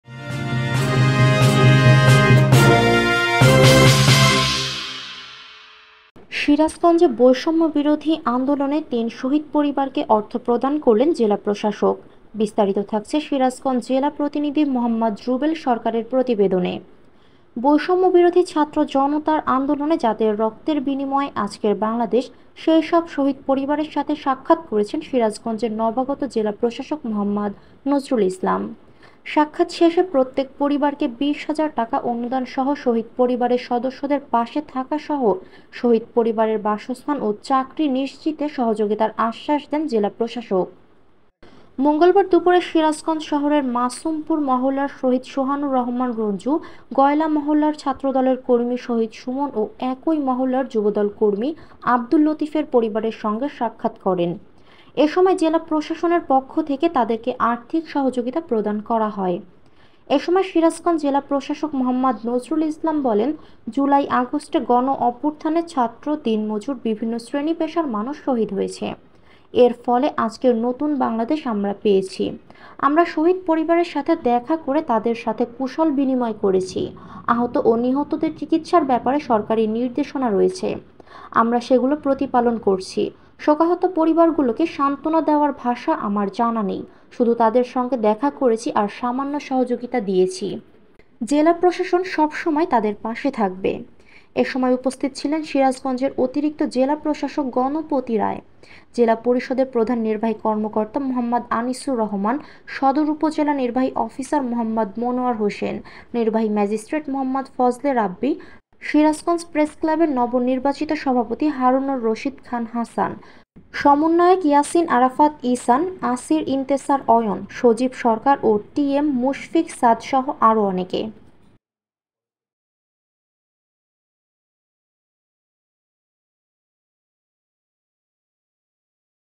সিরাজগঞ্জে বৈষম্য বিরোধী আন্দোলনে ৩ শহীদ পরিবারকে অর্থ প্রদান করলেন জেলা প্রশাসক || Channel 23
news today